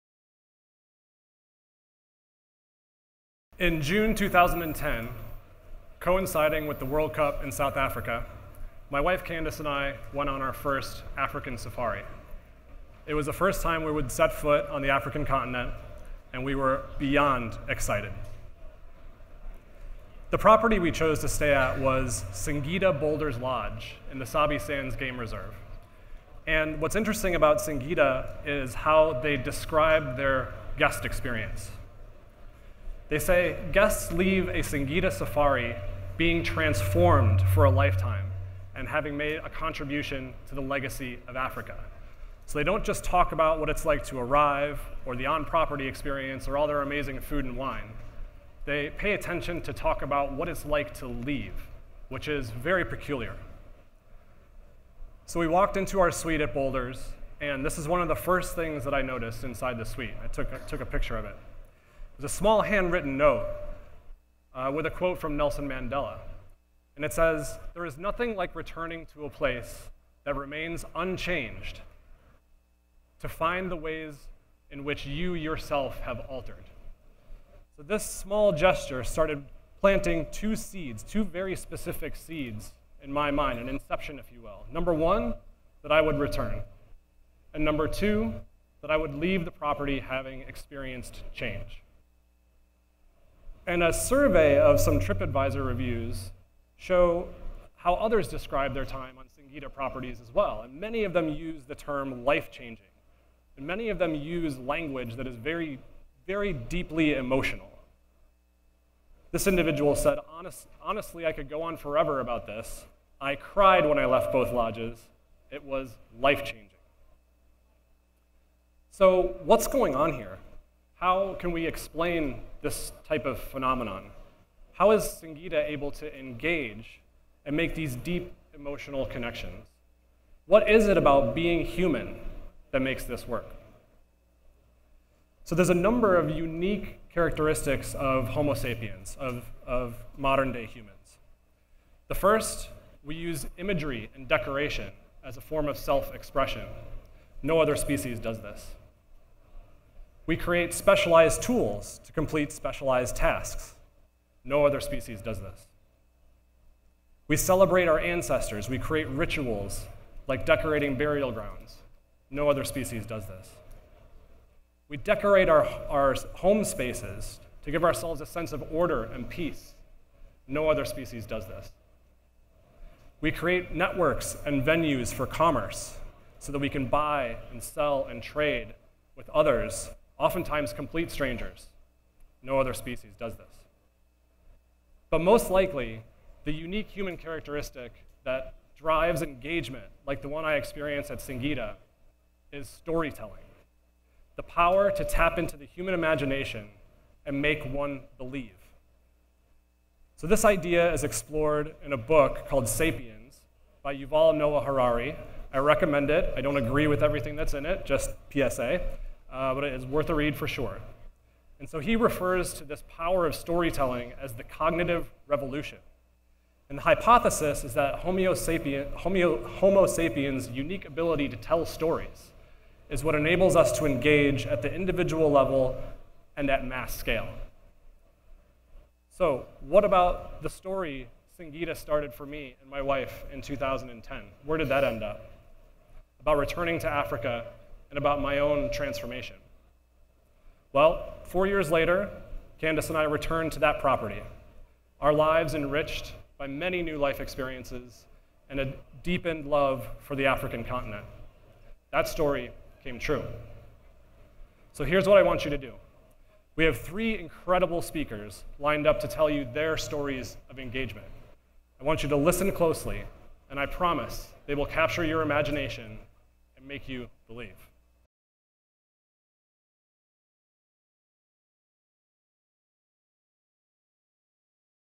Driving customer engagement starts with storytelling | Paul Heckel at Solstice FWD 2018
What does the book Sapiens and customer engagement have in common? Paul Heckel, VP of Customer Experience at Solstice, points to human's unique ability to tell a story.